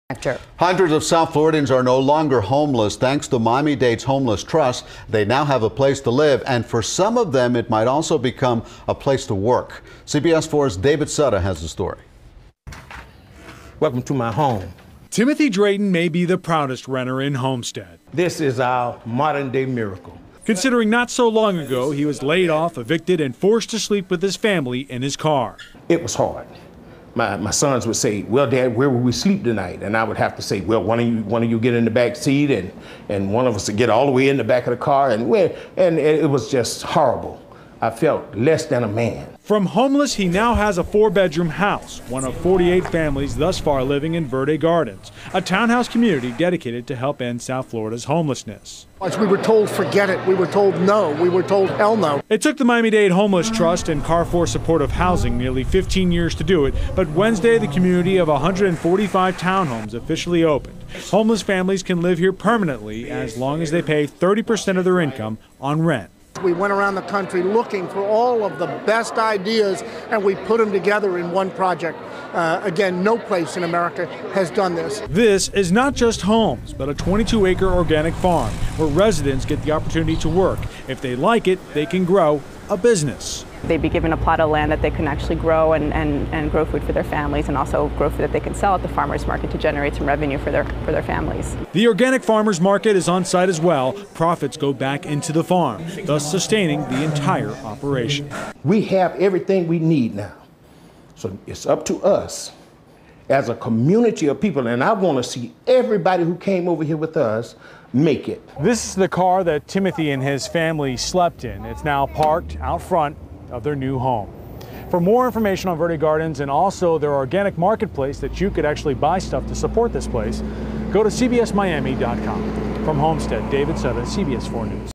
CBS 4 Miami: Innovative Housing Community Opened for Homeless in Homestead, FL
Timothy Drayton may be the proudest renter in Homestead.

"This is our modern day miracle," said Drayton as he showed off his new home at Verde Gardens in Homestead.

Considering not so long ago he was laid off, evicted, and forced to sleep with his family in his car he certainly has some a long way.

"It was hard. My sons would say 'Dad where will be sleep tonight. Well one of you get in the backseat and one of us would get all the way in the back of the car.' It was just horrible. I felt less than a man," said Drayton.

From homeless he now has a four-bedroom house. Drayton was one of 48 families who have moved into Verde Gardens, a townhouse community dedicated to helping end South Florida's homelessness.

Ron Book, chairman of the Miami-Dade Homeless Trust, said it hasn't been easy.

"We were told 'forget it'. We were told 'no'. We were told 'hell no'. We were told 'we are not going to let you do it'. We stayed focused, we stayed committed, we said 'yes we can' because nothing is impossible," Book said.

It took the Homeless Trust and Carrfour Supportive Housing nearly 15 years to do it but Wednesday the community of 145 townhomes officially opened. Homeless families can live here permanently as long as they pay 30% of their income on rent.

"We went around the country looking for all the best ideas and we put them together into one project. Again no place in America has done this," Book said.

There is more there though than just townhouses.

Next door is a 22 acre organic farm where residents will get the opportunity to work. If they like it they can grow a business.

"They will be given a plot of land that they can actually grow. Grow food for their families and grow food they can sell at the farmers market to generate money for their families," explained Stephanie Berman of Carrfour Supportive Housing.

The organic farmers market on the site is already bringing in money. Profits go back into the farm thus sustaining the entire community.

"This is about a second chance. It's about a new opportunity," Berman said.

Drayton agrees.

"We have everything we need now. So it's up to us and I want to see everyone who came here with us make it somewhere in life," said Drayton.

The car that Timothy and his family slept in, a beat up old Ford Explorer is now parked out front of their new home, and he couldn't be happier.